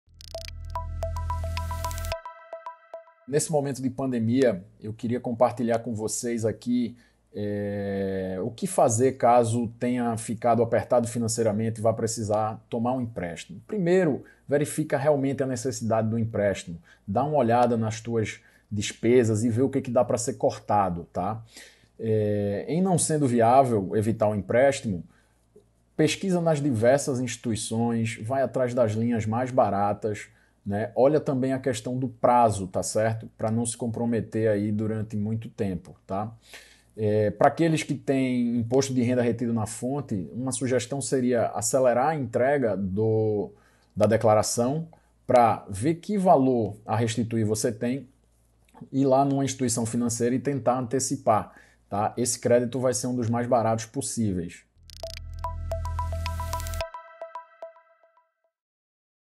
Saiba que fazer caso necessite de um empréstimo | Planejar
O mundo está mudando, e agora?

Paulo Marostica, CFP® e embaixador da Planejar em Pernambuco orienta o que fazer, caso precise realizar um empréstimo.